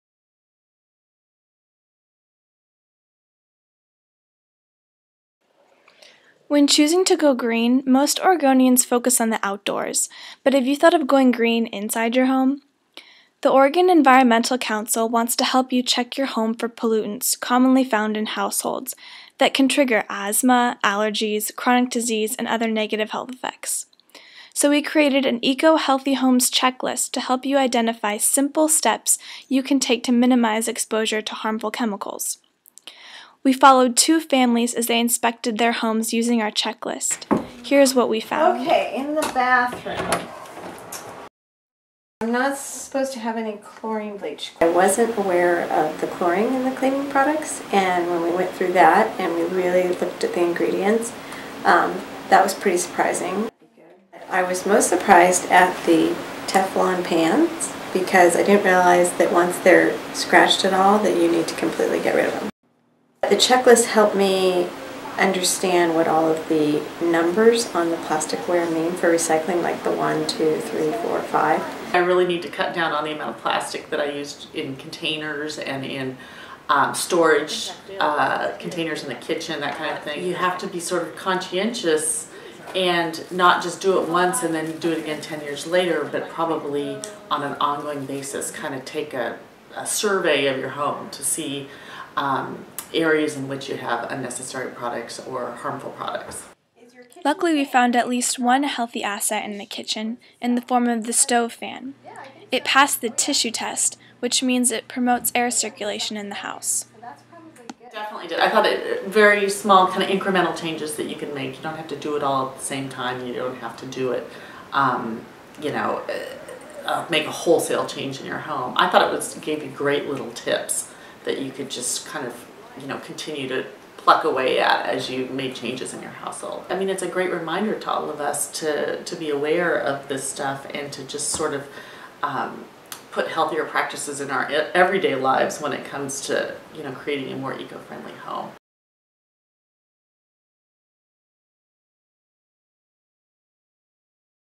Has your home had an eco-healthy checkup?
Two families walk through their homes and find ways to create a healthier environment.